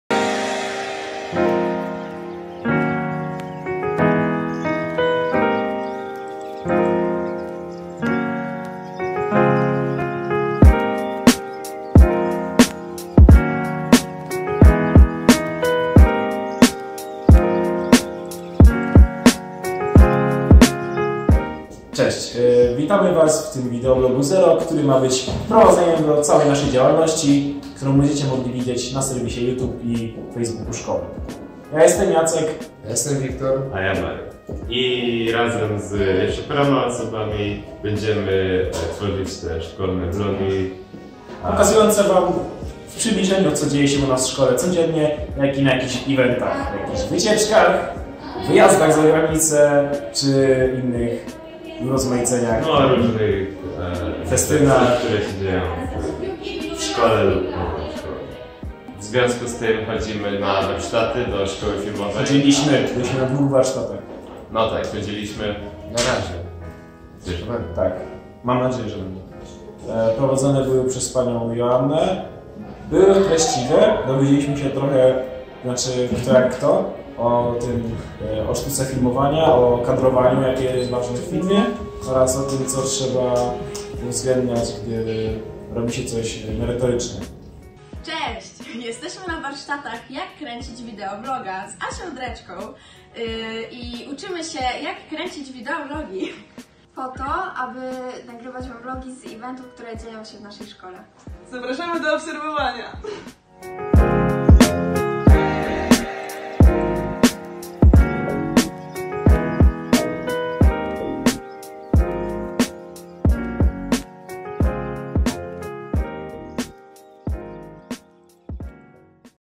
Vlog 0
Inauguracja szkolnego vloga, który tworzą licealiści Zespołu Szkół Jezuitów. Zapraszamy!